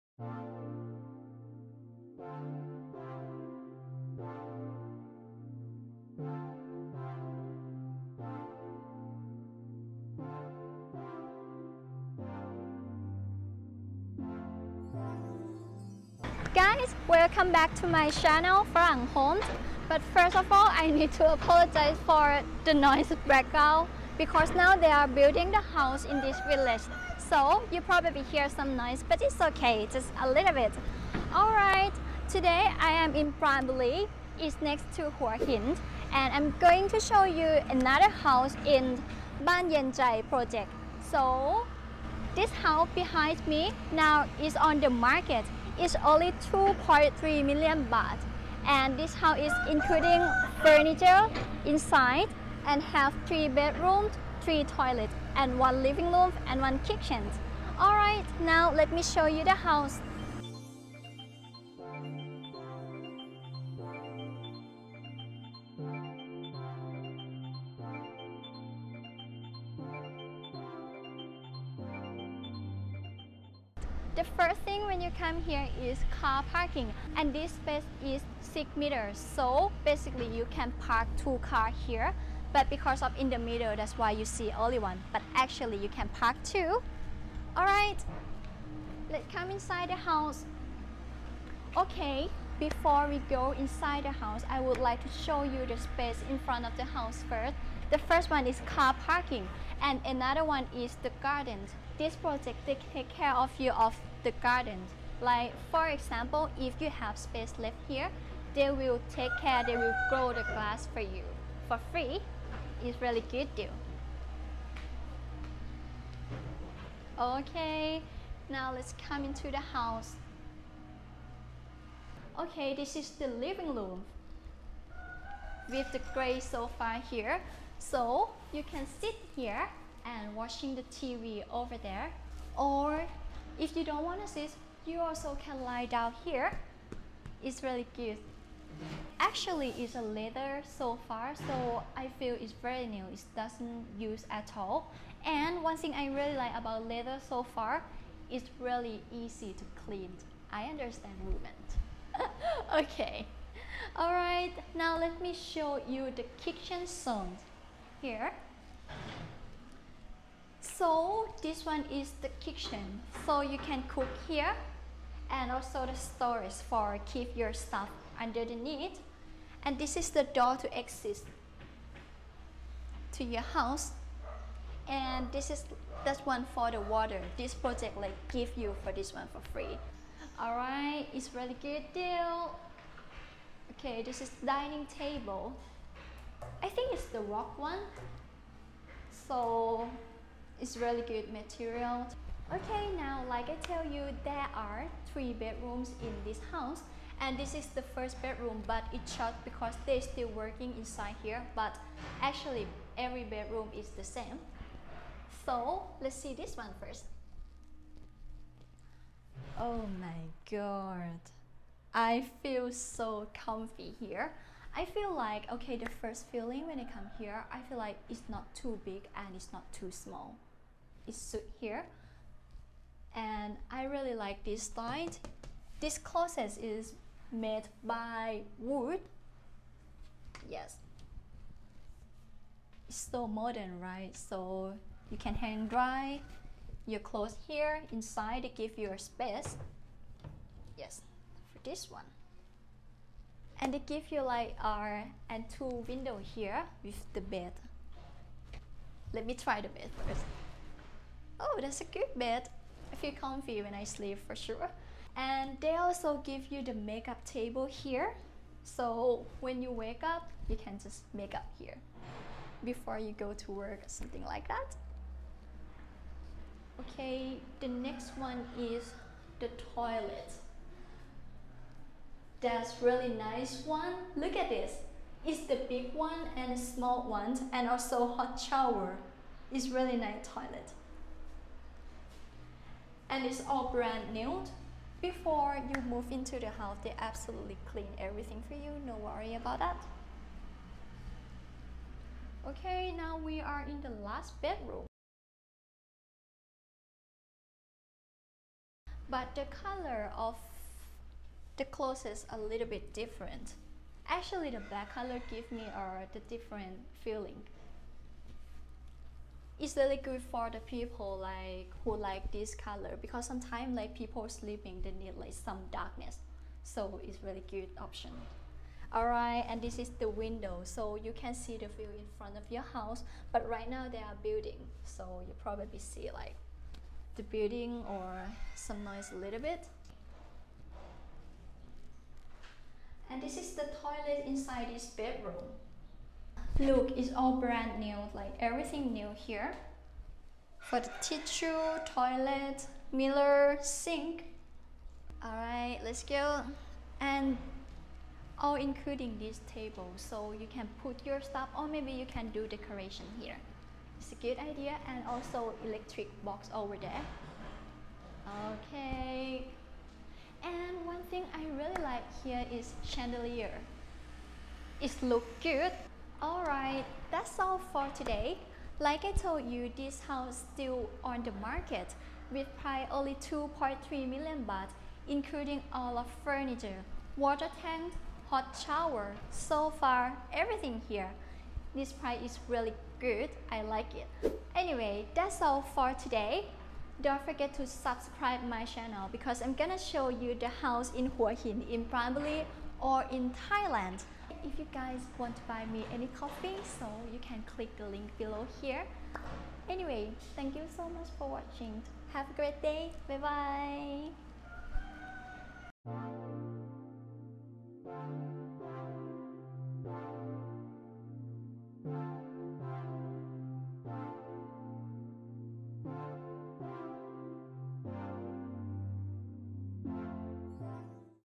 2,300,000 THB ($67,000) Home for Sale in Hua Hin, Thailand (2025)
► Welcome to a new build house in Hua Hin, Pran Buri, Thailand! The price of this house is 2,300,000 THB or 67,000 USD.

► Don't forget to become a channel member if you want to support us and gain exclusive perks! Just click 'Join' below the video,

Rumble: @faranghomes
Threads:
LINE: @faranghomes

***Please note: we are NOT a real estate agency. We handle marketing ONLY.***